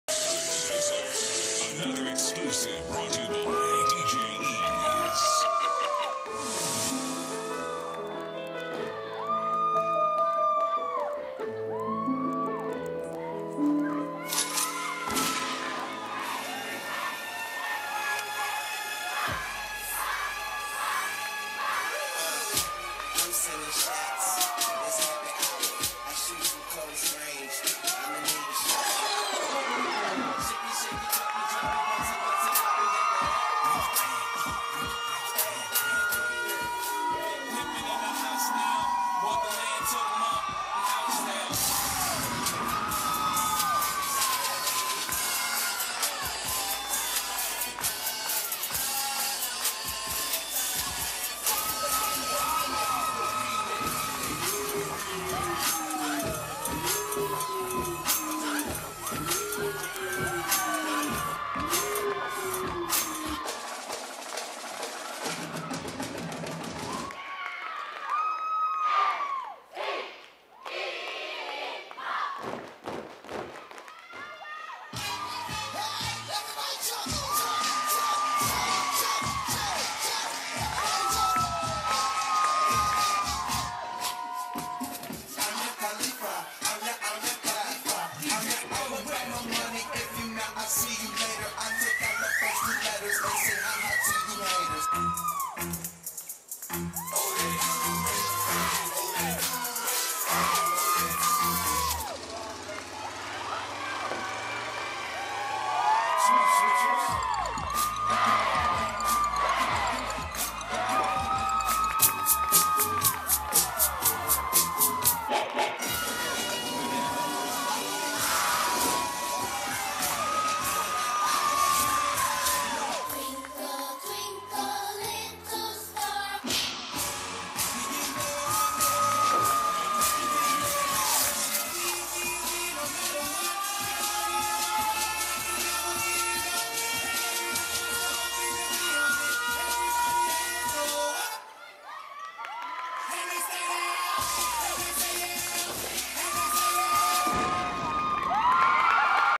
Midwest Cheer Elite: Sr. Coed Hip Hop Worlds 2010 5th place
Dance Worlds 2010: "Midwest Cheer Elite"

Senior Coed Hip Hop: 5th place

Choreographer: Brooke Boling